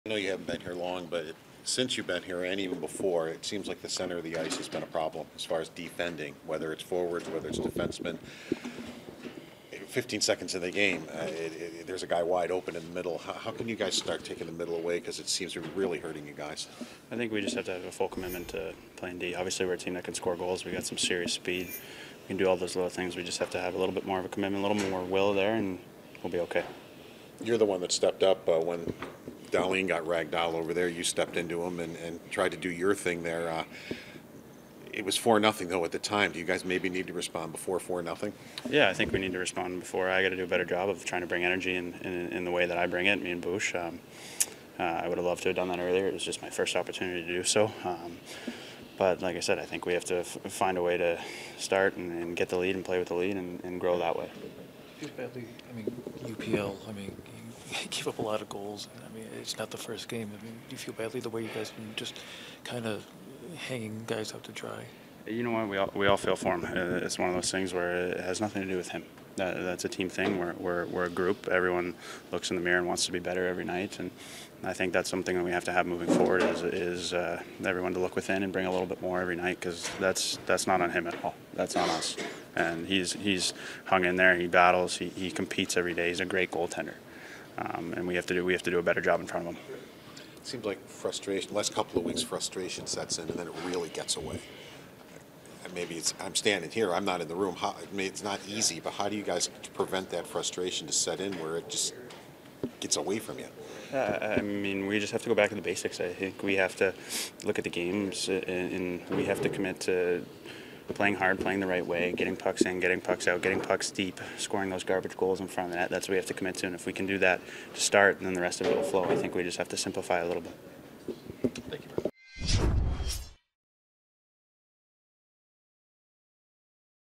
"Need More Of A Commitment To Play D" | Riley Stillman After Buffalo Sabres Loss To Boston
Buffalo Sabres defenseman Riley Stillman addresses the media following the Sabres loss to the Boston Bruins on March 19, 2023.